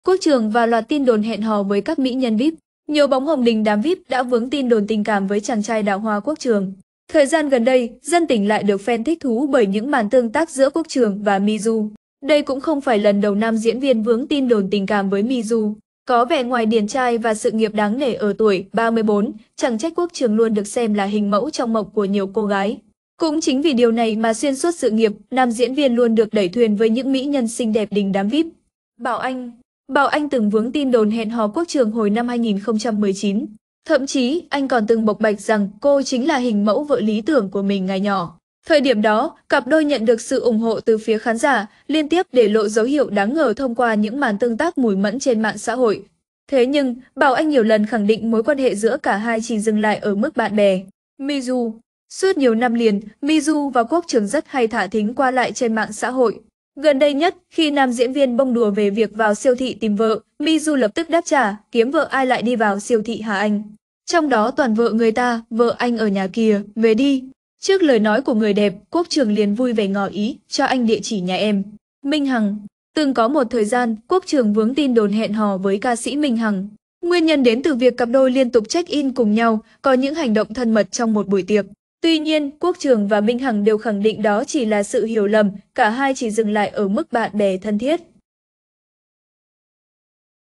Quốc Trường và loạt tin đồn hẹn hò với các mỹ nhân Vbiz
Nhiều "bóng hồng" đình đám Vbiz đã vướng tin đồn tình cảm với chàng trai đào hoa Quốc Trường.Suốt nhiều năm liền, Midu và Quốc Trường rất hay "thả thính" qua lại trên MXH. Gần đây nhất, khi nam diễn viên bông đùa về việc "vào siêu thị tìm vợ", Midu lập tức đáp trả: "Kiếm vợ ai lại đi vào siêu thị hả anh. Trong đó toàn vợ người ta, vợ anh ở nhà kìa, về đi...". Trước lời nói của người đẹp, Quốc Trường liền vui vẻ ngỏ ý: "Cho anh địa chỉ nhà em".
Từng có 1 thời gian, Quốc Trường vướng tin đồn hẹn hò với ca sĩ Minh Hằng. Nguyên nhân đến từ việc cặp đôi liên tục check-in cùng nhau, có những hành động thân mật trong một buổi tiệc. Tuy nhiên, Quốc Trường và Minh Hằng đều khẳng định đó chỉ là sự hiểu lầm, cả 2 chỉ dừng lại ở mức bạn bè thân thiết.
Thời gian gần đây, dân tình lại được phen thích thú bởi những màn tương tác giữa Quốc Trường và Midu. Đây cũng không phải lần đầu nam diễn viên vướng tin đồn tình cảm với Midu. Có vẻ ngoài điển trai và sự nghiệp đáng nể ở tuổi 34, chẳng trách Quốc Trường luôn được xem là hình mẫu trong mộng của nhiều cô gái. Cũng chính vì điều này mà xuyên suốt sự nghiệp, nam diễn viên luôn được "đẩy thuyền" với những mỹ nhân xinh đẹp đình đám Vbiz.
Minh Hằng
Bảo Anh từng vướng tin đồn hẹn hò Quốc Trường hồi năm 2019. Thậm chí, anh còn từng bộc bạch rằng cô chính là hình mẫu vợ lí tưởng của mình ngày nhỏ. Thời điểm đó, cặp đôi nhận được sự ủng hộ từ phía khán giả, liên tiếp để lộ dấu hiệu đáng ngờ thông qua những màn tương tác mùi mẫn trên MXH. Thế nhưng, Bảo Anh nhiều lần khẳng định mối quan hệ giữa cả 2 chỉ dừng lại ở mức bạn bè.
Midu
Bảo Anh